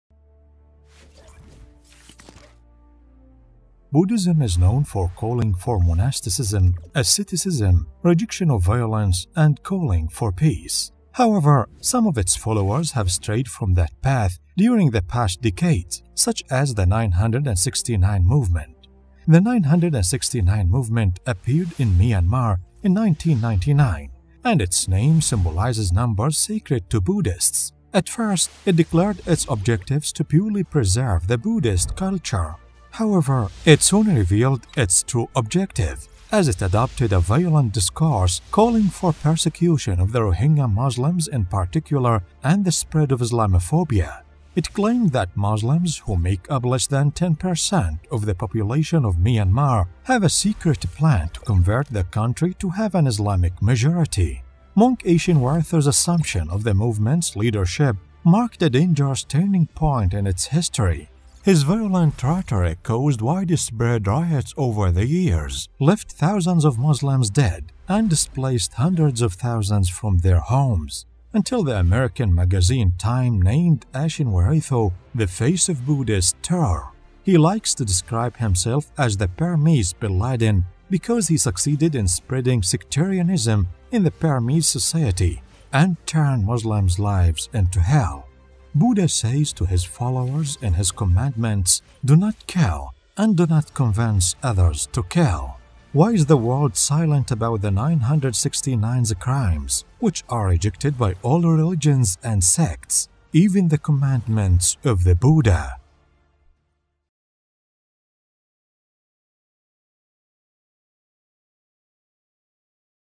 Le mouvement bouddhiste 969
Le mouvement bouddhiste 969 est passé de la promotion du monachisme et de la paix à l'un des groupes terroristes les plus virulents au monde.
Dans ce court métrage, nous révélons comment ce mouvement controversé a transformé la vie des musulmans rohingyas en enfer.